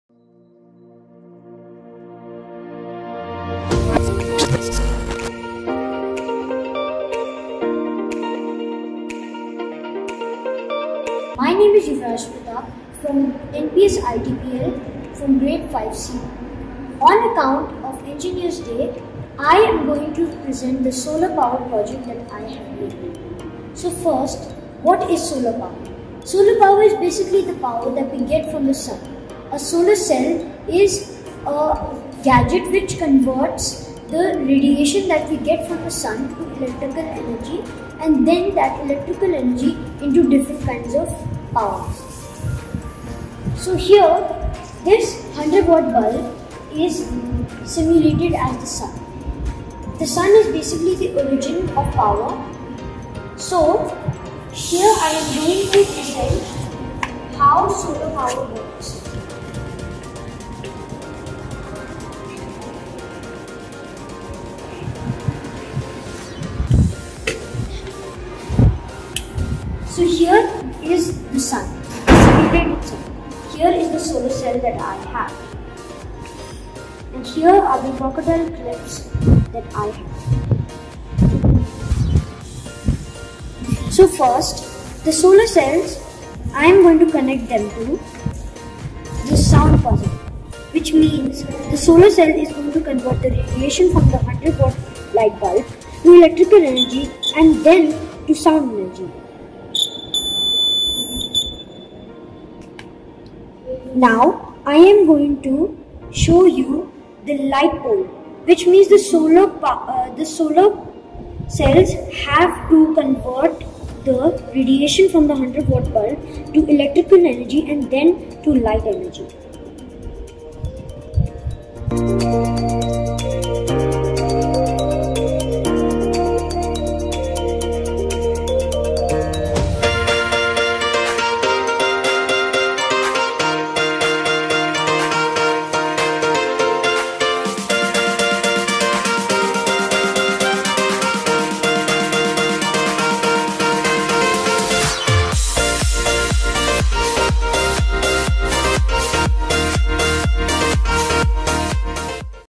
Engineer's Day 2022 (Special Assembly)
On account of Engineer’s day, the school encouraged and appreciated the innovative contributions of our students. Engineer’s day is celebrated on 15th September to commemorate the great achievements of Sir Mokshagundam Visvesvaraya on his birth anniversary. Some students shared their opinion in the form of speeches in the classroom. Few students went a step forward to demonstrate their projects in the science lab.
On the whole students enjoyed displaying and sharing knowledge with each other.